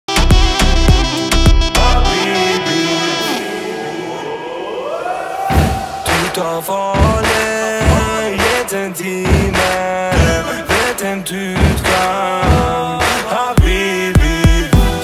For the love❤️ Indonesian red winged dragonfly
Neurothemis terminata is a species of dragonfly in family Libellulidae. Neurothemis terminata is a widespread and often common species which can occur in man-made habitats, Male N. terminata have red colour on its body and wings, while the female have yellowish colors @gistzillion